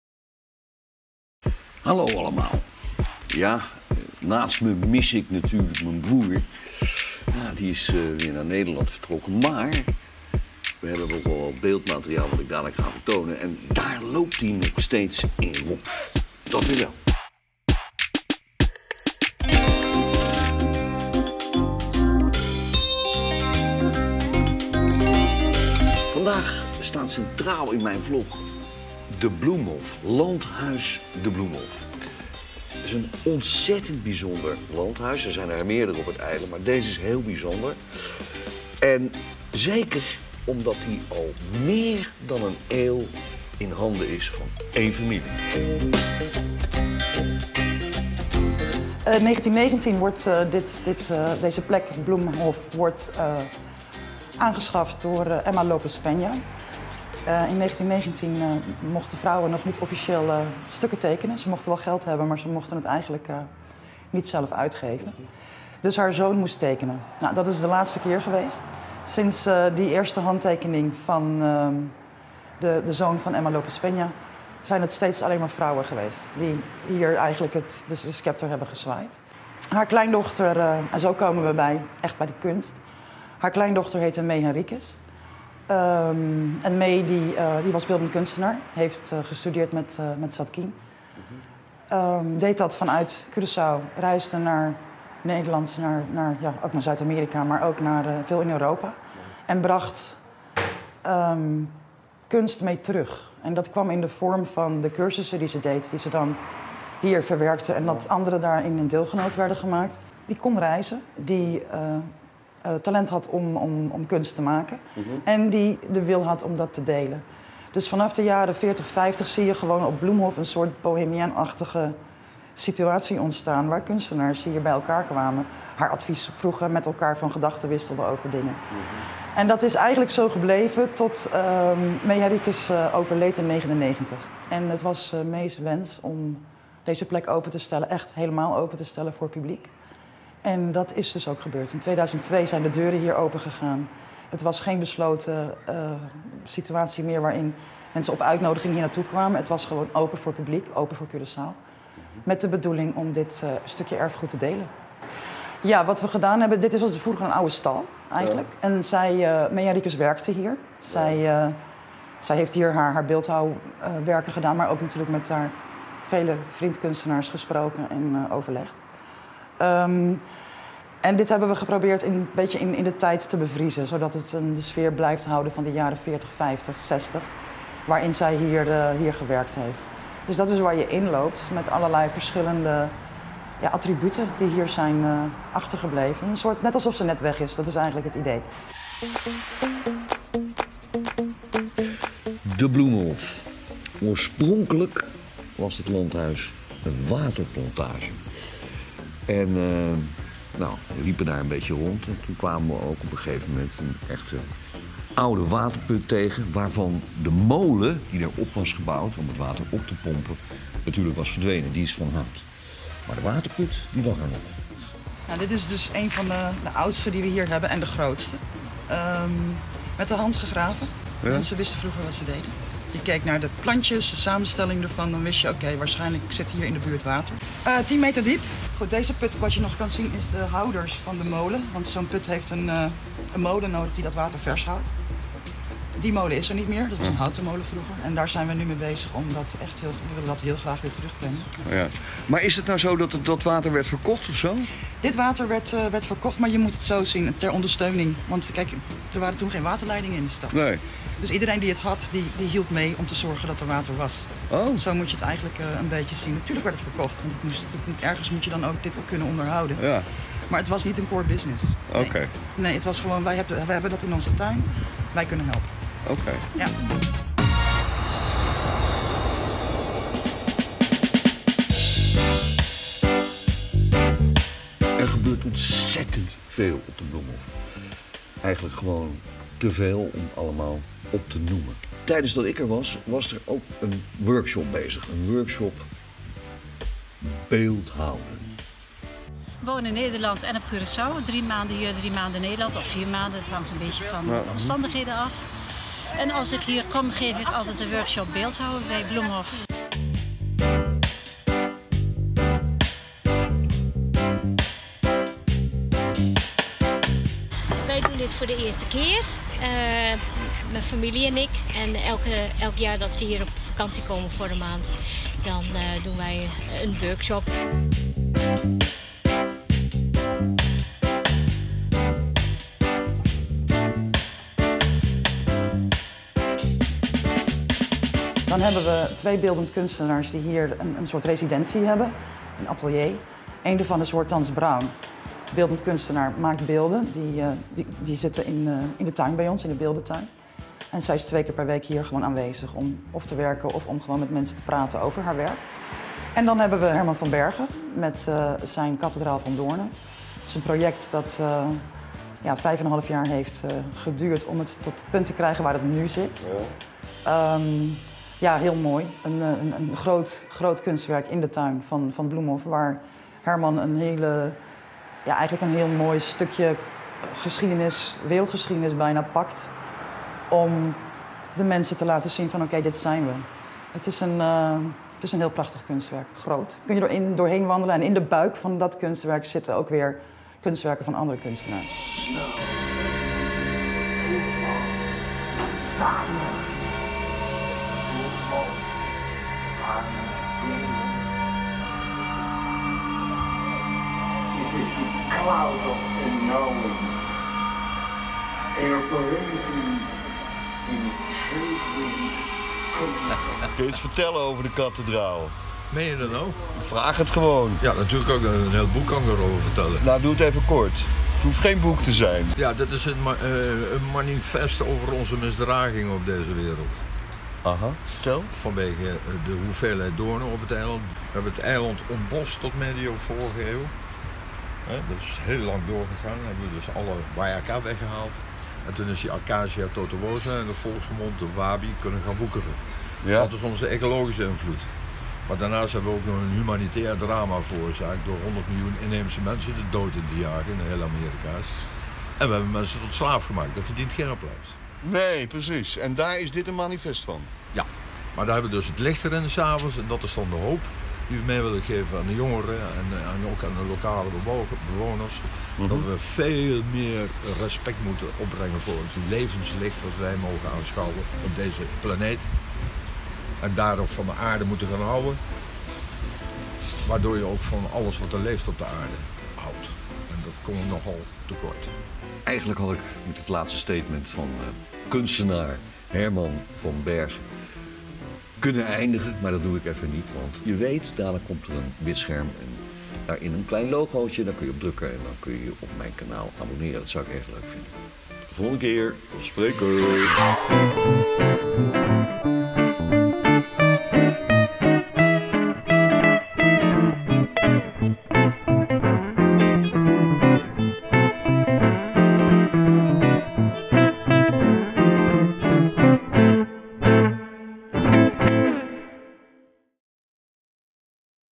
Kunst op Curacao. De Bloemhof. De oudste vlogger van Nederland
De oudste Vlogger van Nederland, Peter Mader, filmde op Landhuis Bloemhof. Als jaren een belangrijk kunstcentrum op Curacao. We krijgen een inkijkje in de geschiedenis van het landhuis en aandacht voor het kunstwerk van Herman van Bergen; the Cathedral of Thorns. De Kathedraal van Doornen. Te bijzonder om te missen als je Curacao bezoekt.